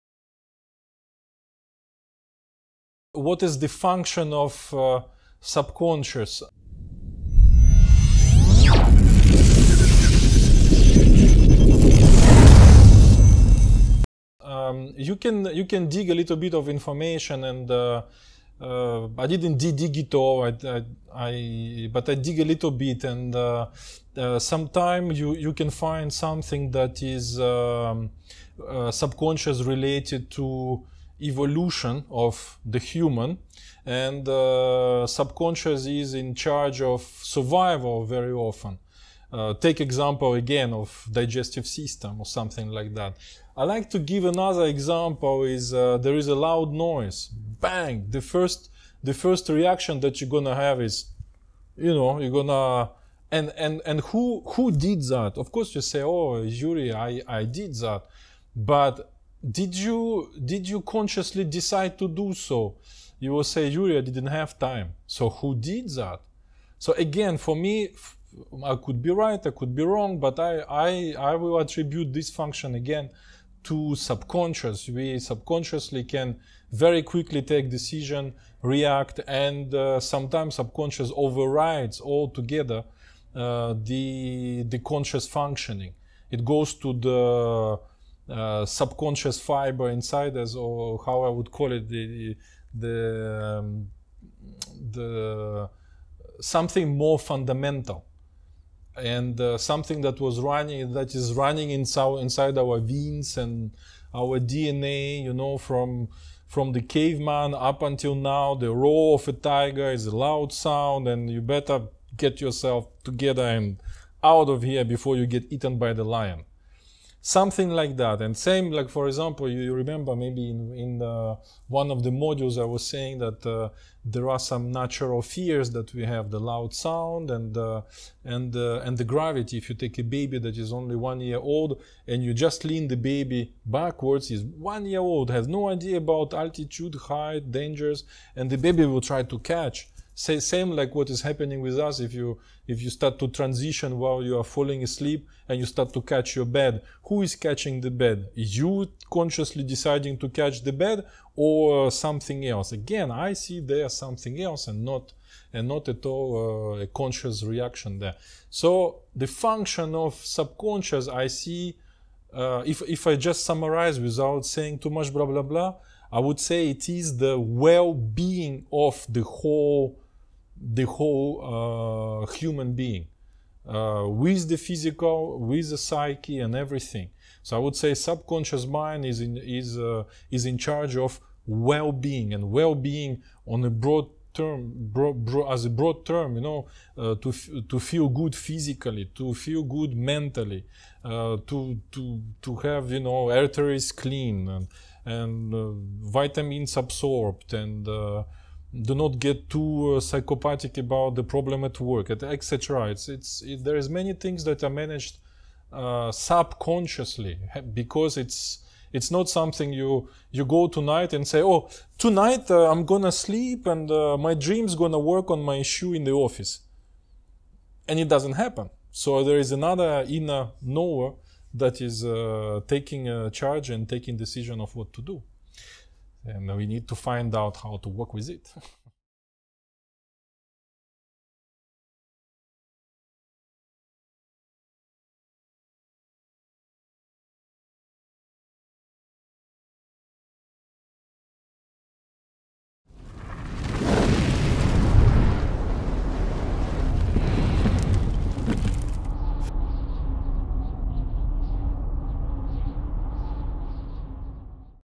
Subconscious Mind - Function & Lucid Dreaming - Astral Projection
Part II of the series (6 more parts to go).

This part will explain the function of the subconscious mind so that you get a better idea of how to apply the cooperation with it in the practice of shifting to Lucid Dreaming and Astral Projection experiences.